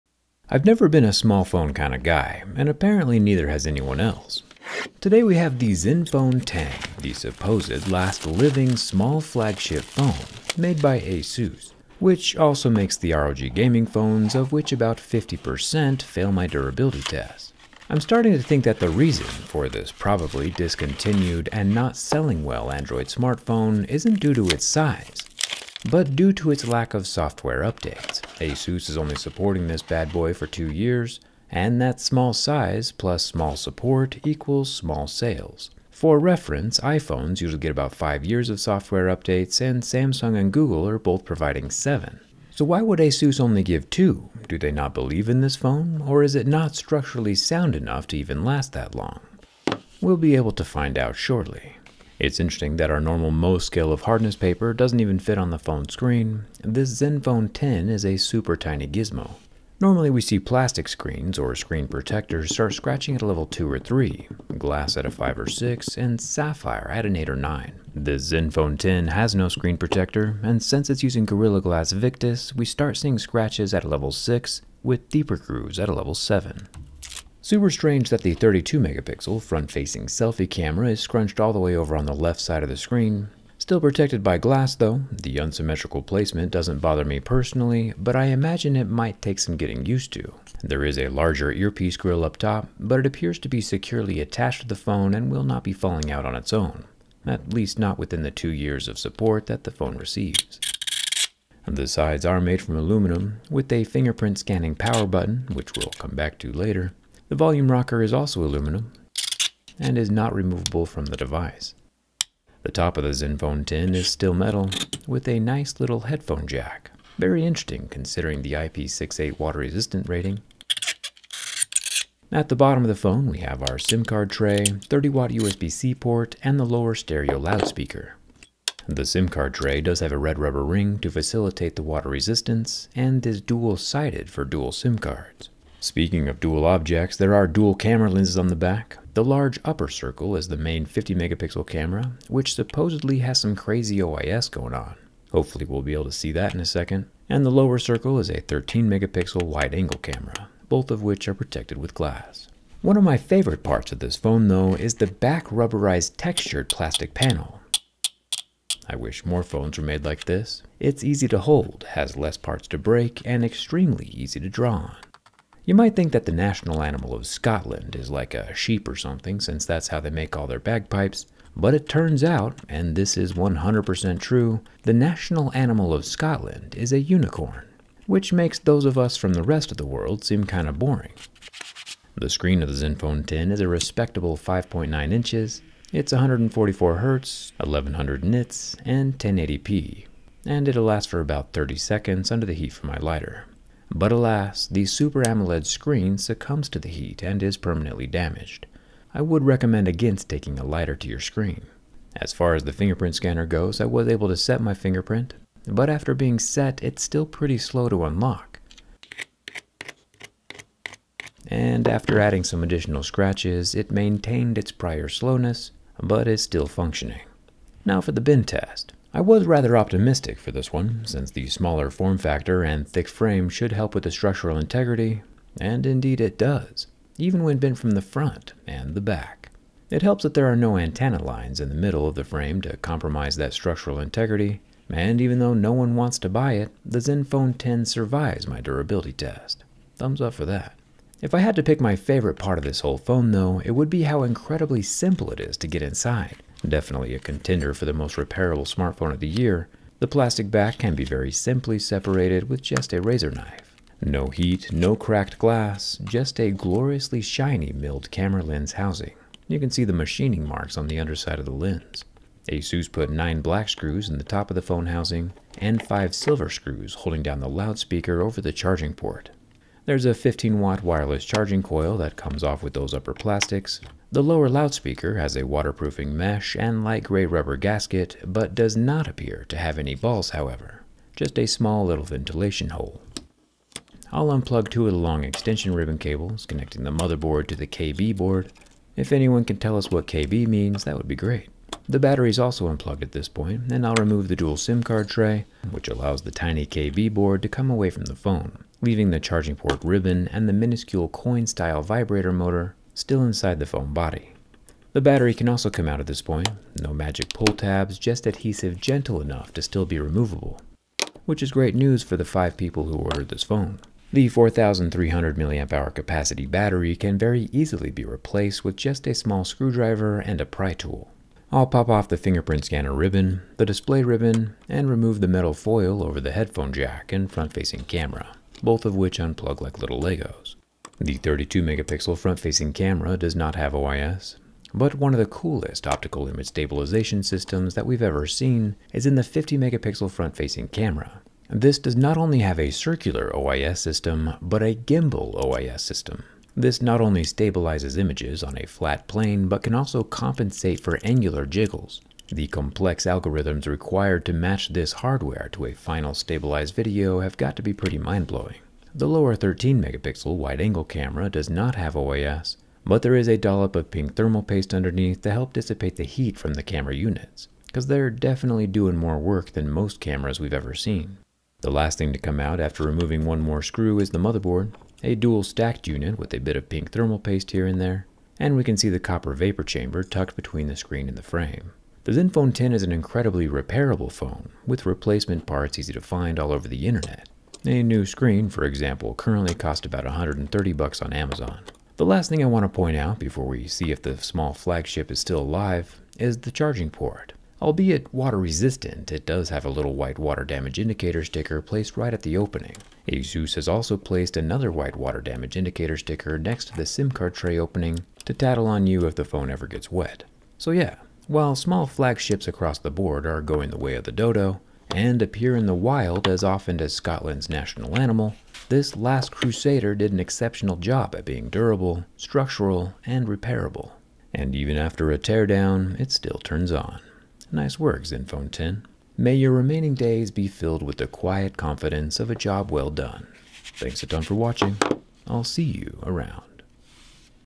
The Last Living Small Flagship - (Rest in Peace)
Today we durability test and take apart the Zenphone 10 the last living (for the moment) smartphone.